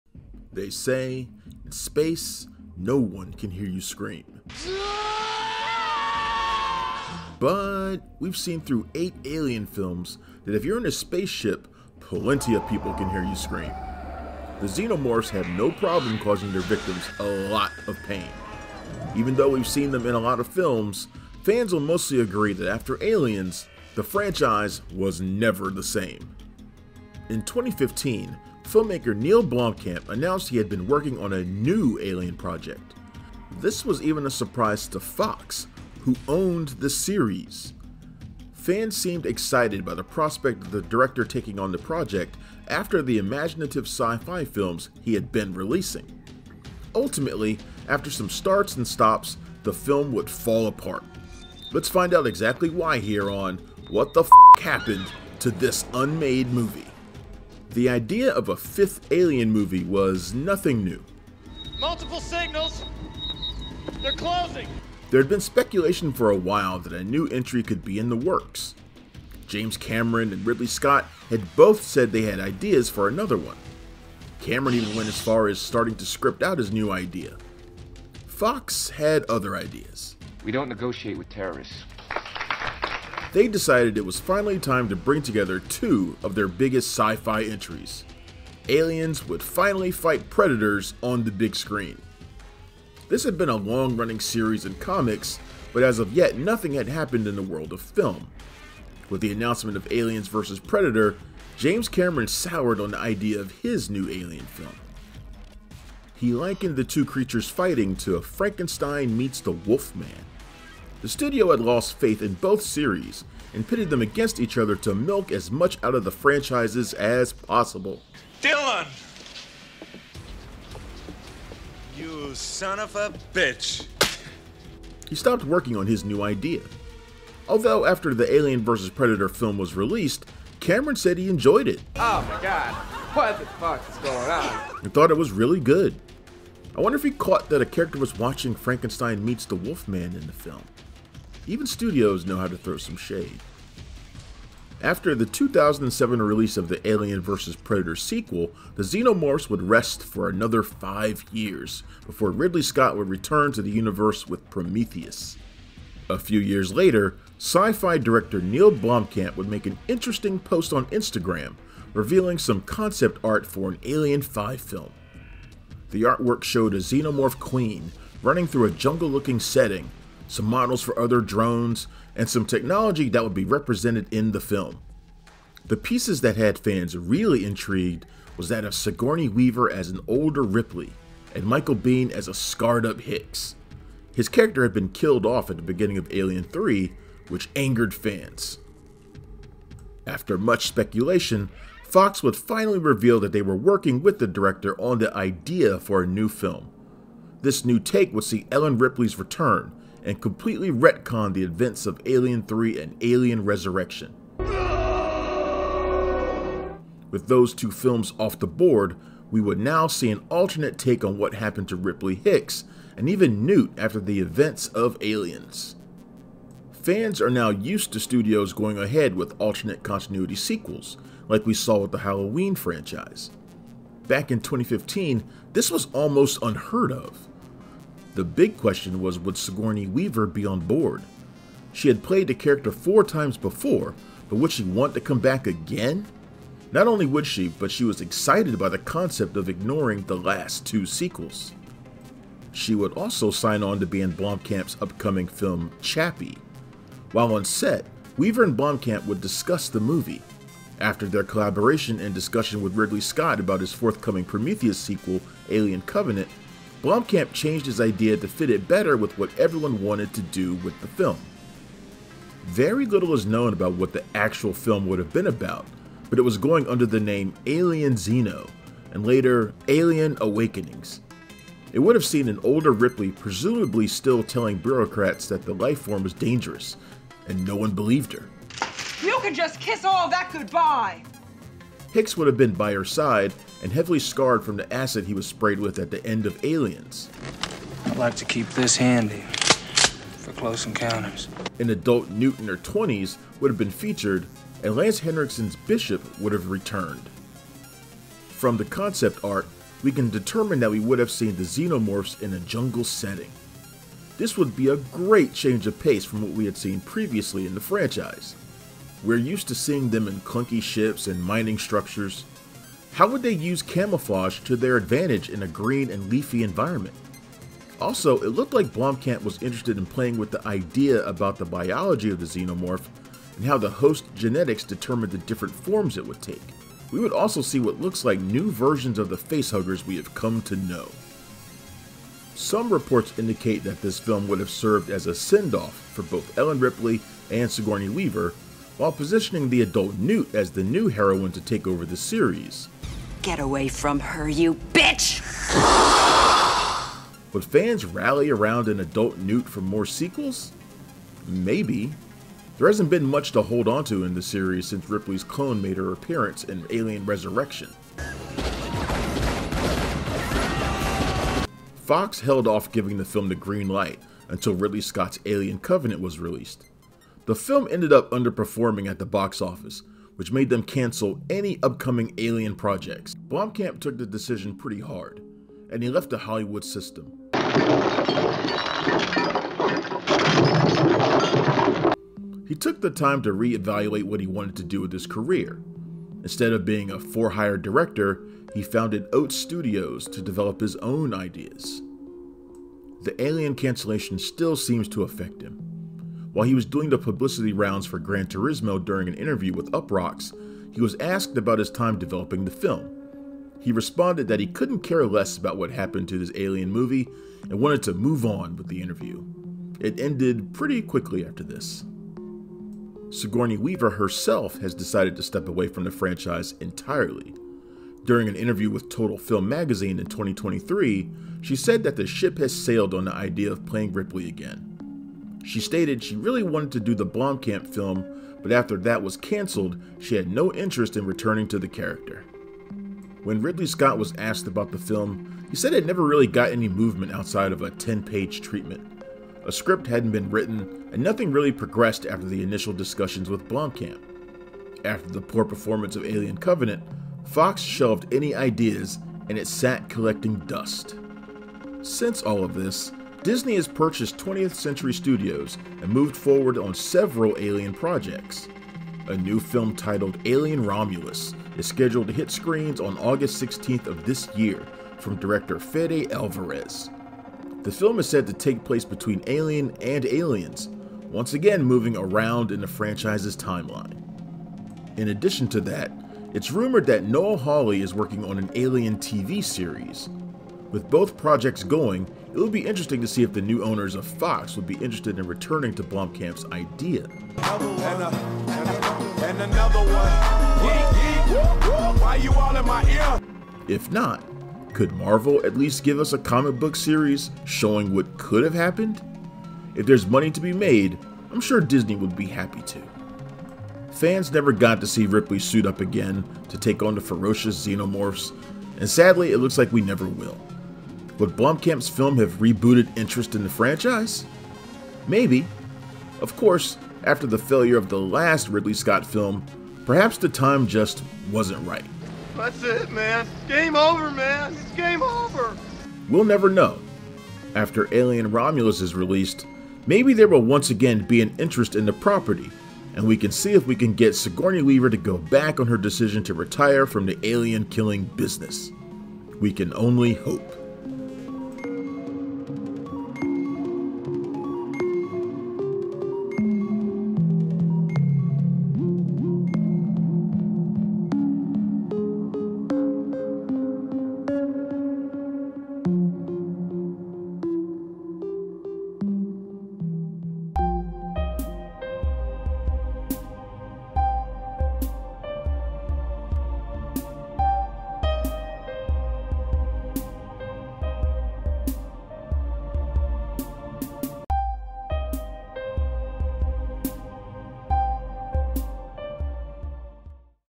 Neill Blomkamp's Alien 5: What Happened To This Unmade Movie?
They say, "In space, no one can hear you scream." But we've seen through eight Alien films that plenty of people can hear you scream if you're in a spaceship. The Xenomorphs have no problem causing their victims a lot of pain. Even though we've seen them in many films, fans will mostly agree that after Aliens, the franchise was never the same.

In 2015, filmmaker Neill Blomkamp announced he had been working on a new Alien project. This was even a surprise to Fox, which owned the series. Fans seemed excited by the prospect of the director taking on the project after the imaginative sci-fi films he had been releasing. Ultimately, Neil Blomkamp's Alien 5 would fall apart after some starts and stops. Let's find out exactly why here on What Happened To This Unmade Movie